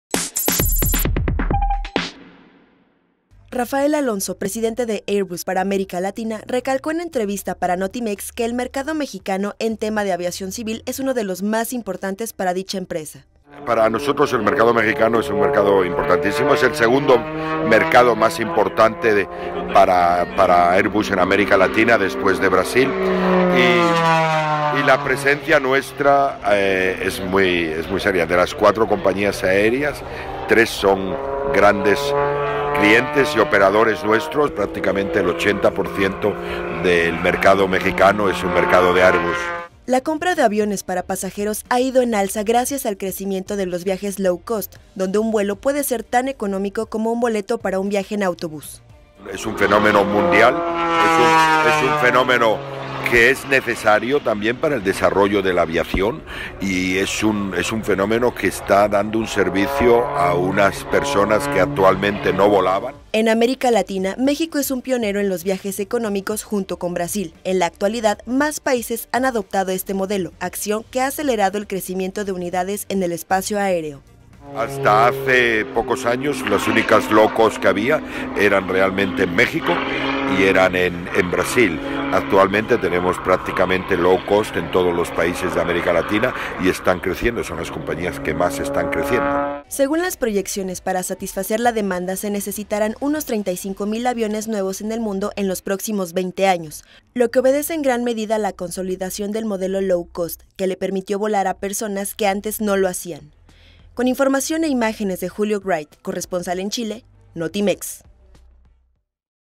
Valora Airbus mercado mexicano de aviones de pasajeros
En América Latina, México es un pionero en los viajes económicos junto a Brasil, en la actualidad, más países han adoptado este modelo, acción que ha acelerado el crecimiento de unidades en el espacio aéreo.